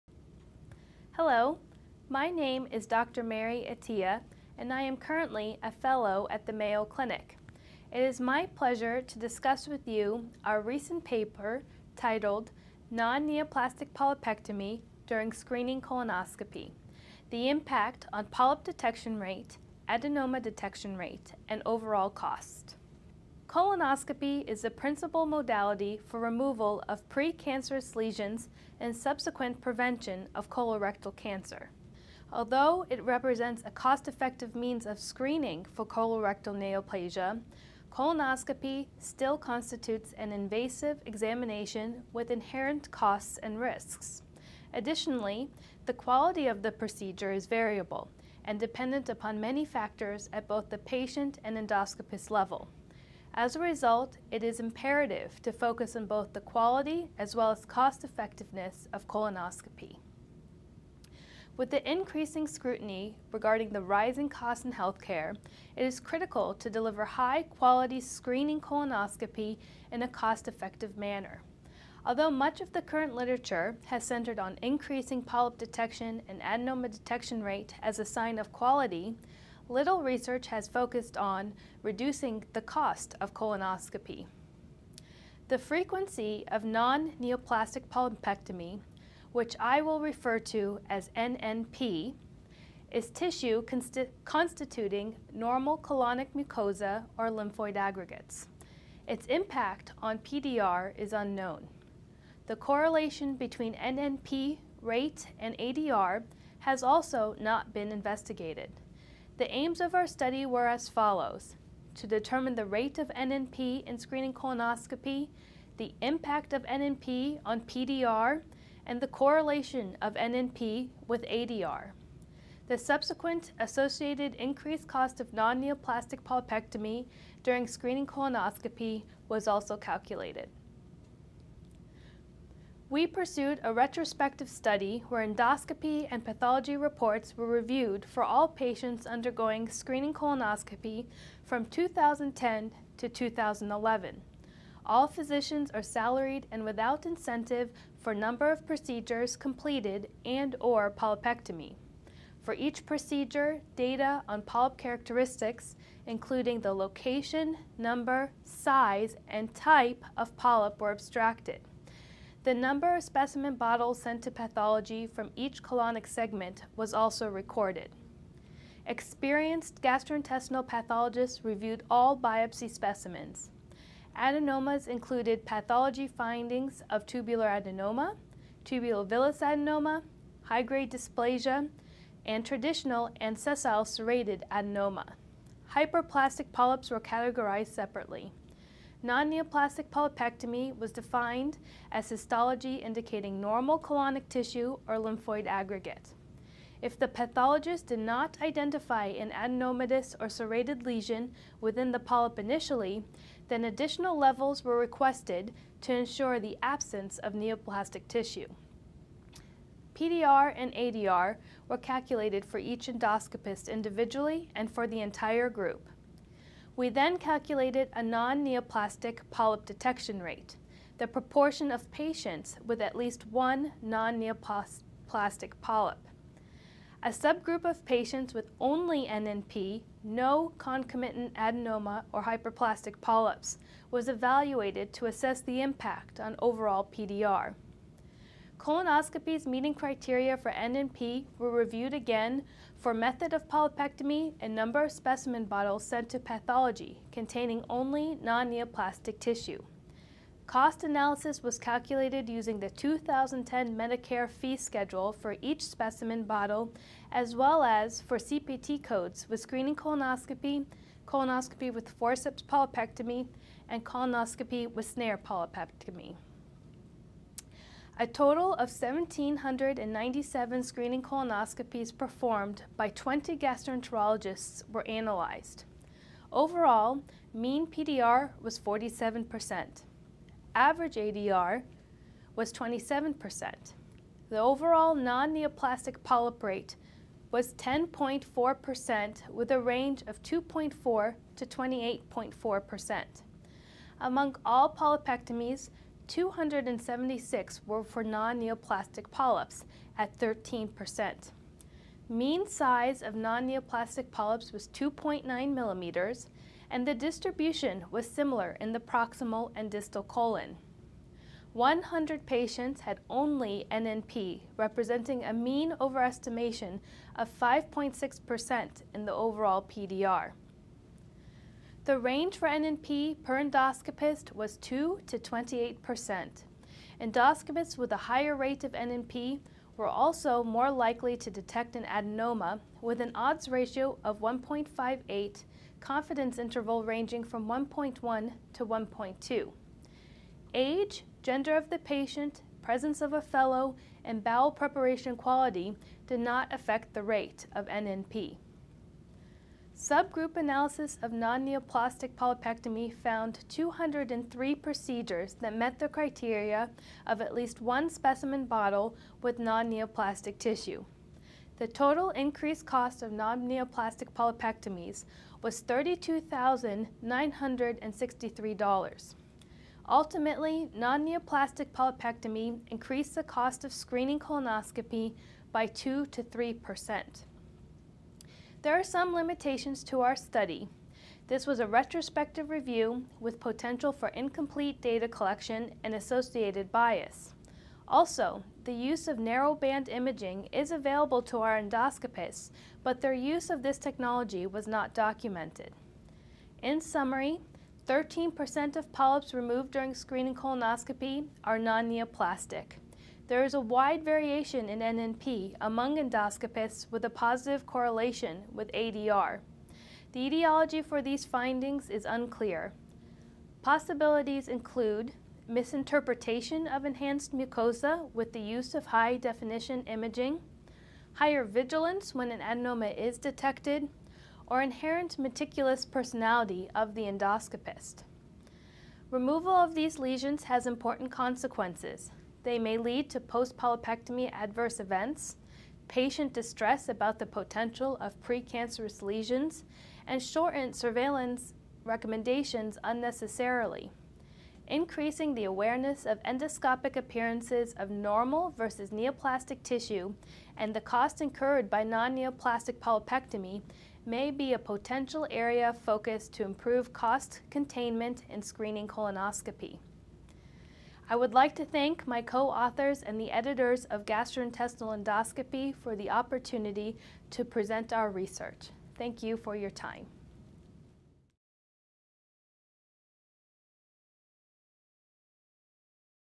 2015 August Author Interview Series-Mary Atia
Dr Mary Atia discusses her article, "'Non-neoplastic polypectomy during screening colonoscopy: the impact on polyp detection rate, adenoma detection rate, and overall cost" from the August issue.